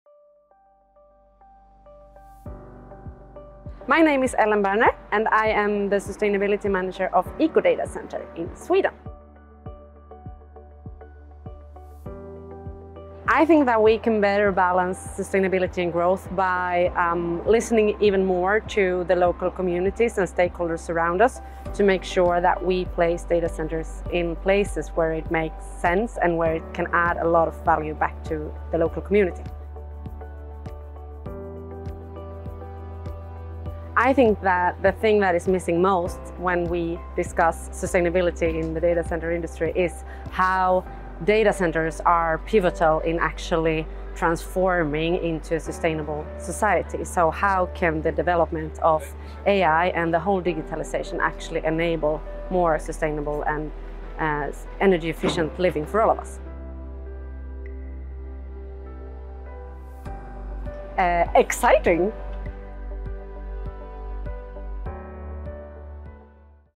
Ellen Berner on the Role of Data Centers in Building a Sustainable Society | Datacloud Energy & ESG
Ellen Berner, Sustainability Manager at EcoDataCenter, shares how the data center industry can become a powerful force for sustainable development. In the latest episode of our spotlight series, Ellen emphasizes the importance of engaging local communities and placing infrastructure where it adds meaningful value.